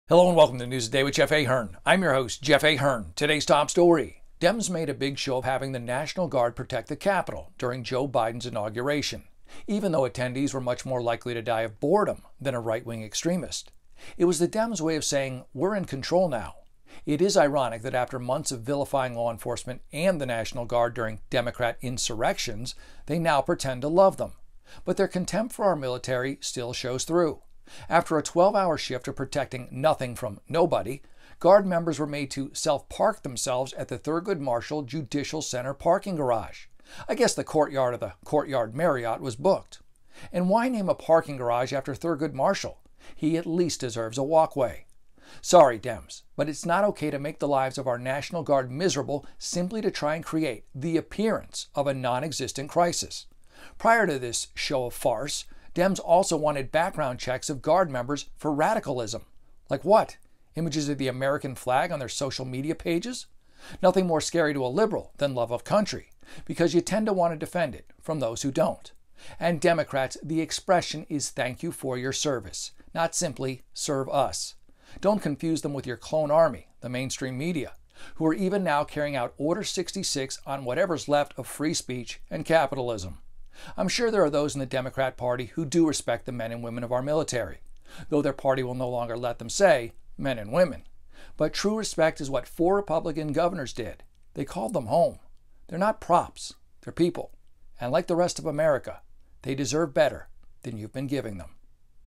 Free Parking!
Dems make a show of force or farce.  Using the men and women of our military as props in their quest for legitimacy.   Ohh look at all the troops we need for the non existent threat.  Meanwhile the left has 5 or 6 insurrections since Biden takes office and there is zero main stream media coverage.